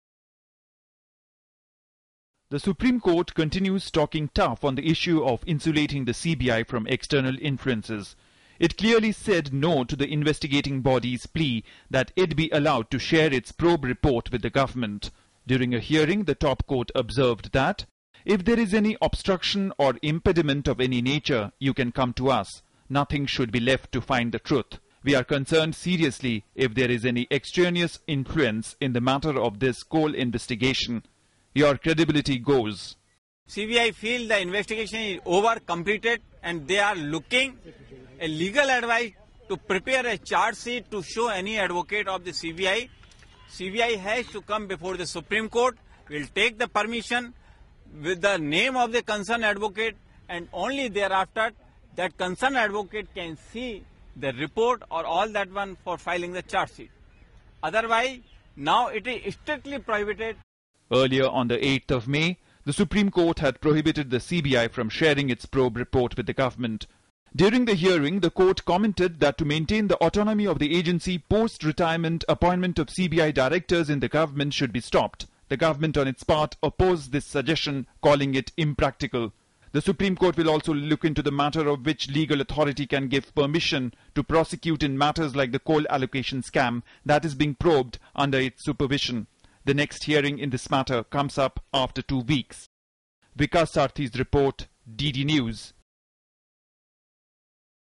News Night:SC tells CBI, leave no stone unturned in Coal block case
The Supreme Court told the Central Bureau of Investigation (CBI) on Wednesday, that nothing should be left and no stone must be left unturned in finding out the truth in the coal block probe.